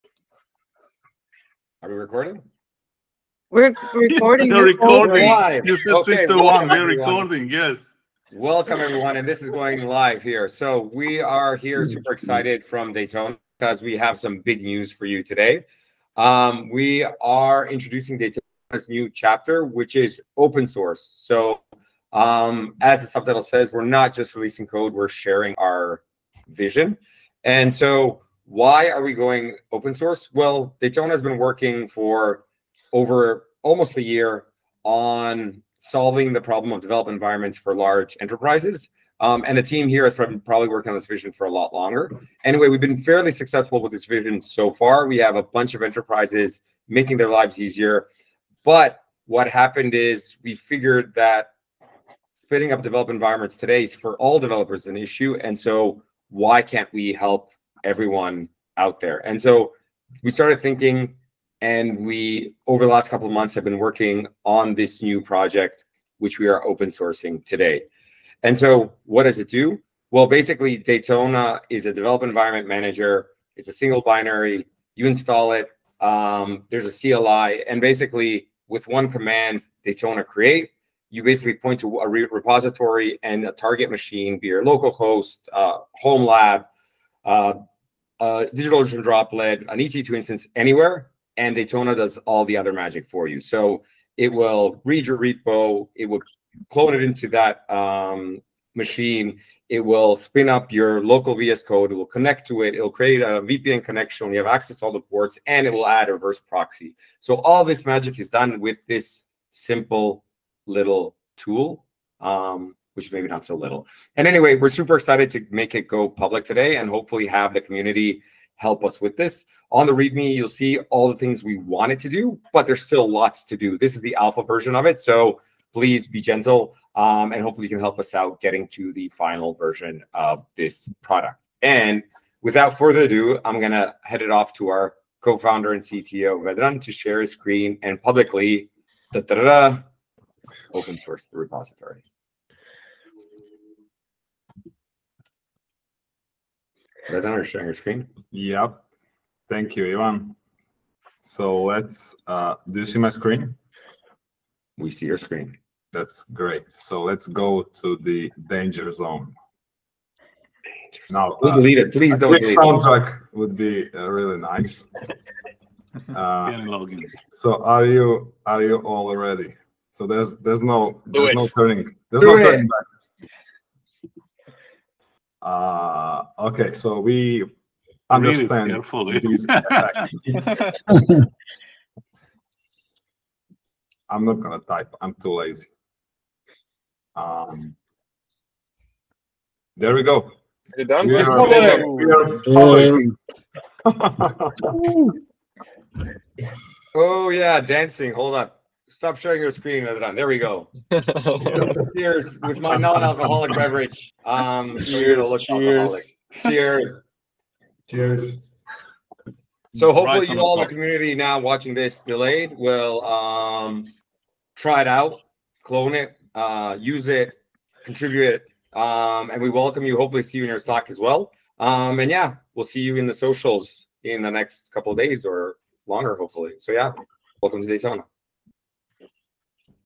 Daytona Goes Open Source: Unveiling the Future of Dev Environments! 🚀 | Daytona Release Event
🎉 The wait is over! The Daytona Team is thrilled to bring you a groundbreaking announcement LIVE – Daytona has officially gone open source! 🎉

In this special livestream event, join us as we unveil the alpha version of Daytona, a powerful development environment manager that's about to revolutionize how developers work. With a simple command, Daytona creates an environment that connects your local VS Code to a target machine, manages VPN connections, and much more – all with the ease of a single binary!

🔍 Why go open source? It's not just about releasing code; it's about sharing a vision that empowers EVERY developer. We've seen the challenges firsthand and have made strides with large enterprises. Now, it's time to extend that success to the broader development community.

💡 Dive into the live launch with our team and witness the public release of our repository. Feel the excitement as we hit the 'Open Source' button and watch as Daytona transforms from a private project to a community-driven innovation.

📢 We're eager to collaborate with you! Let's shape the future of development environments together.